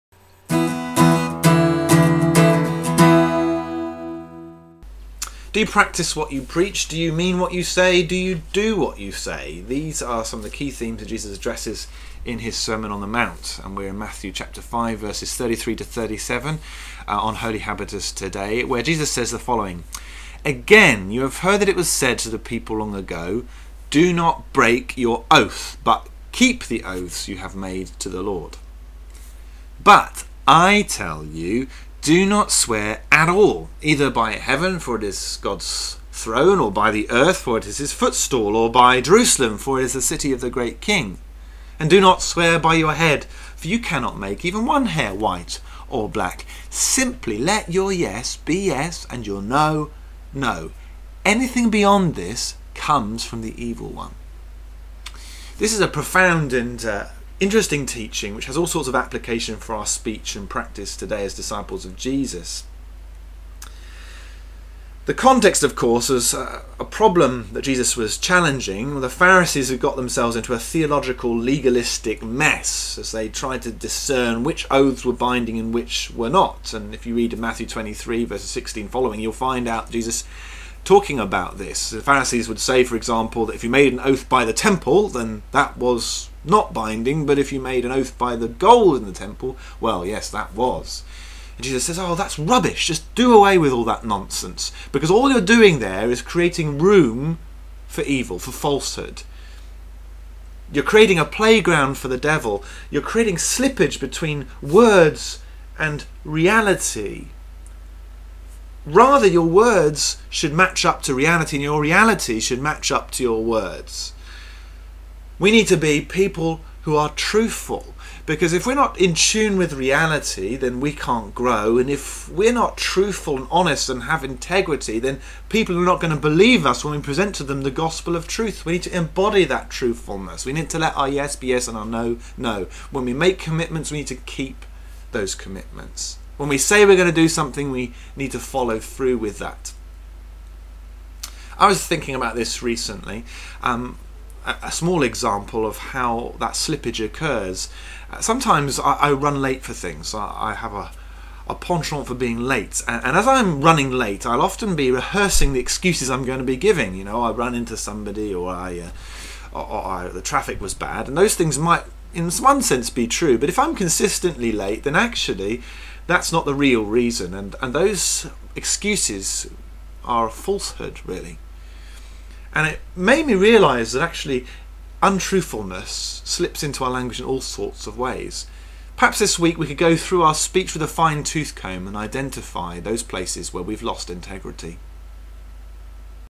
truthful speech
In Matt 5:33-37 Jesus teaches about oaths and integrity. This has a lot to say to us today in our culture where words are often cheap and commitments card-board cut-outs blown over by a stiff breeze. How can we sift through our speech and check for truth slippage? How can we ensure our yes is yes and our no, no?